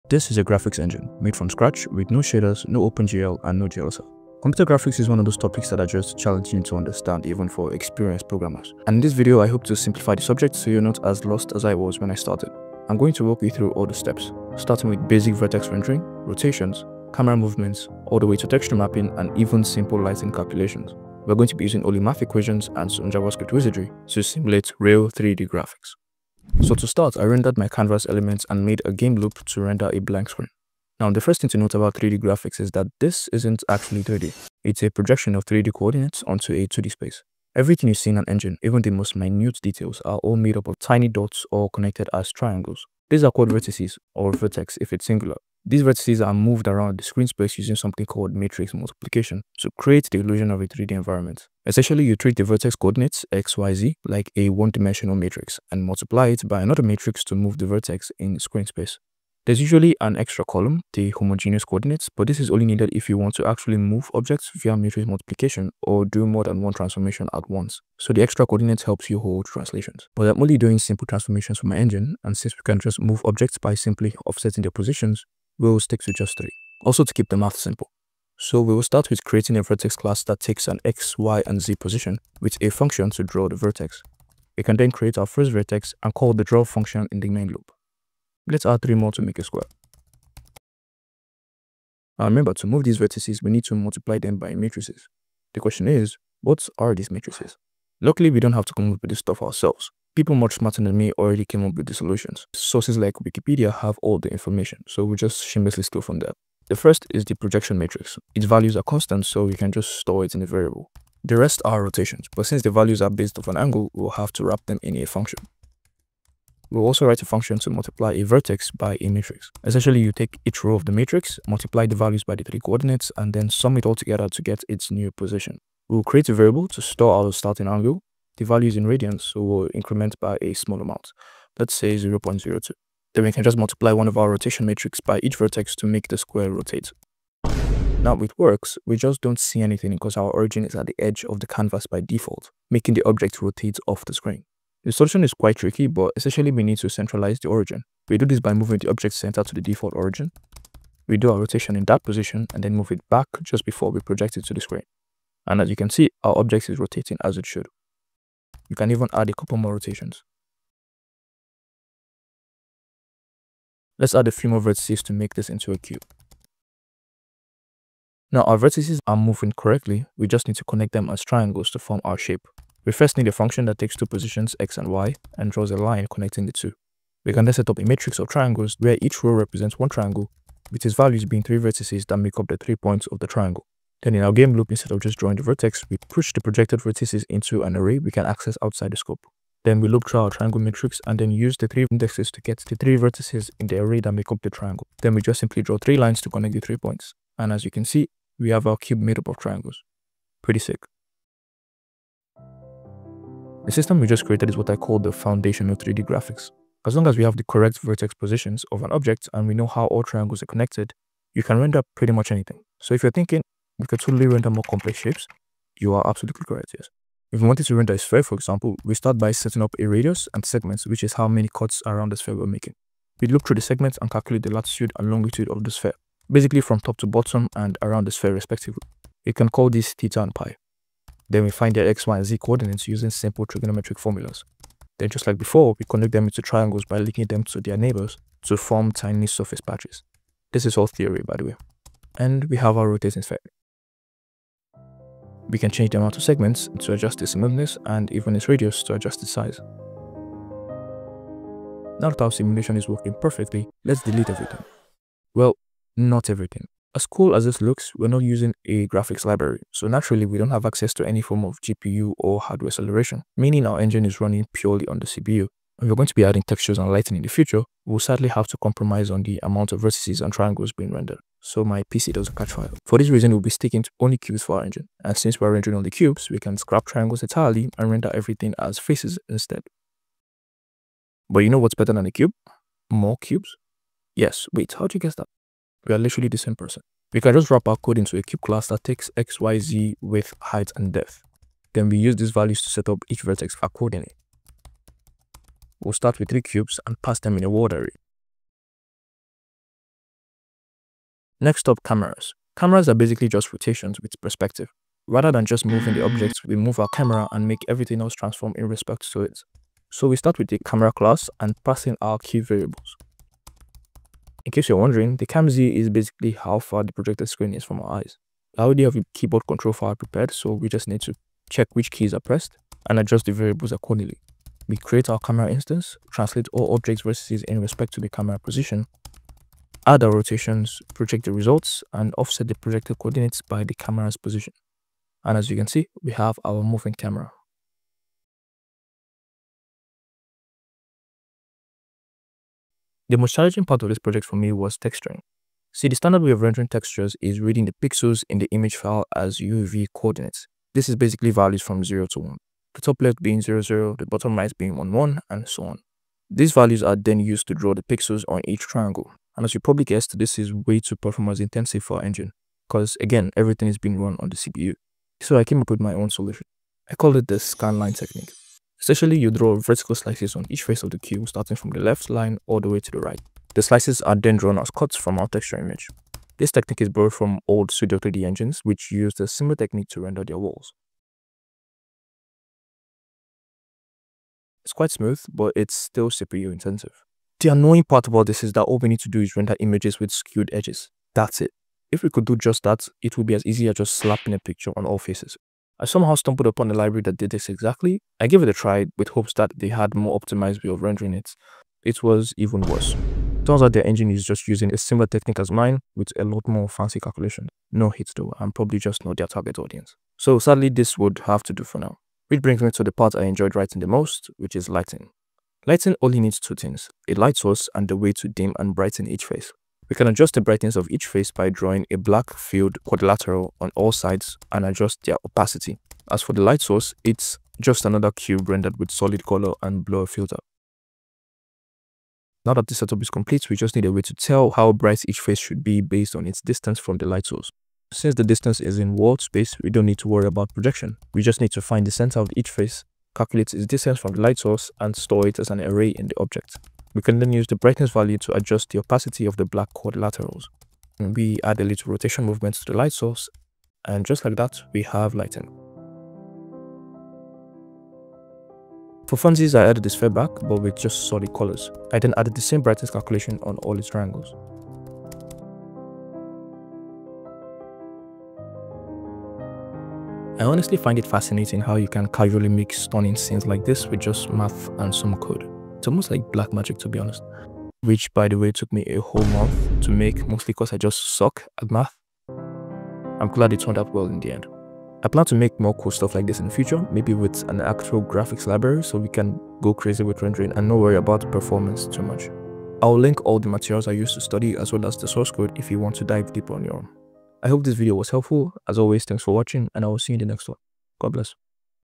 Building a Graphics Engine from Scratch (NO OpenGl, NO Shaders)
Credit to @kevkev-70 for some of the clips